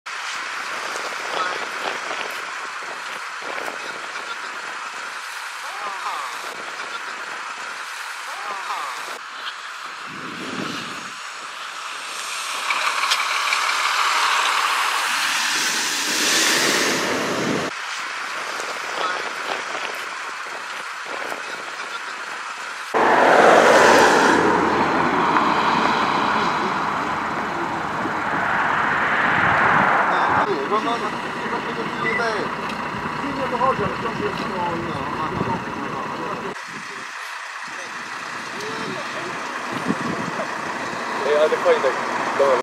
Gm. Władysławów: Peugeotem do rowu. Kobieta w ciąży trafiła do szpitala
Dwie kobiety, w tym jedna w ciąży, zostały poszkodowane w zdarzeniu drogowym, do którego doszło dzisiaj, 3 stycznia, około 9:10, na trasie Turek – Władysławów.

W miejscowości Mariantów, jadący od strony Turku peugeot 206 prawdopodobnie nie dostosował prędkości jazdy do warunków panujących na drodze, w wyniku czego wpadł w poślizg, a następnie do pobliskiego rowu.